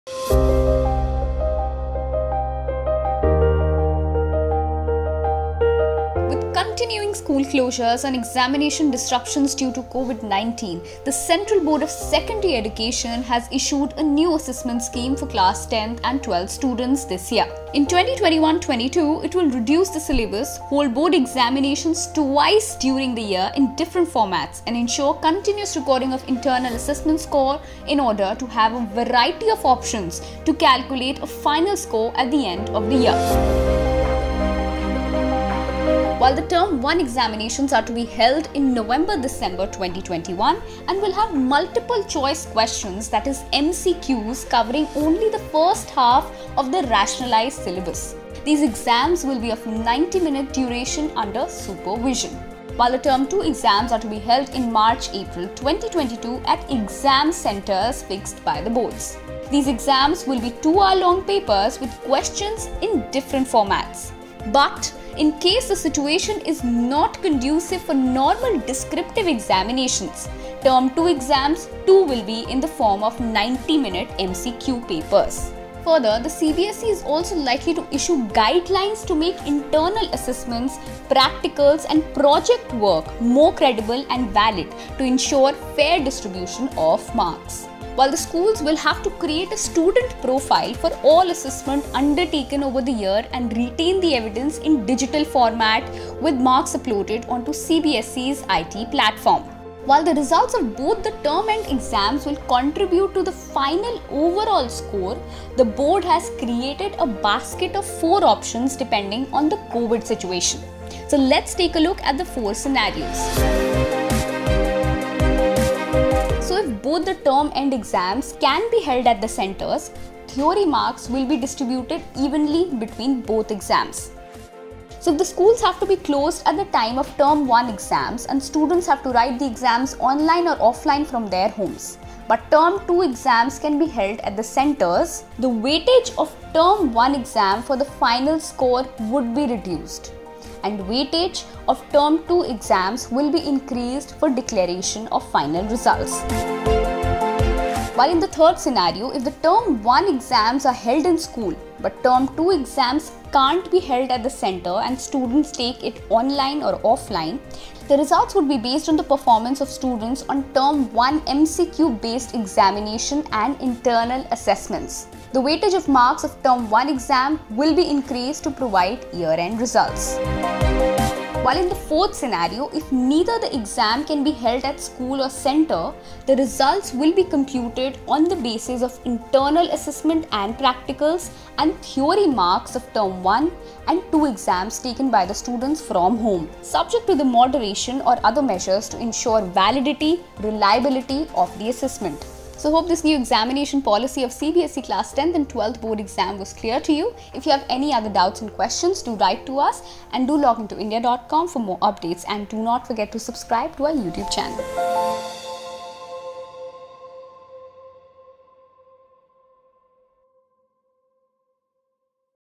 CBSE Class 10, 12 Board Exams 2021-2022: All you need to know about two-exam policy | Latest News
The syllabus of both the board exams will be rationalised and will be conveyed to the schools in July 2021 itself. The paper pattern of the board exams will also be changed with more objective questions apart from long-answer questions in the exams. Watch video for more details.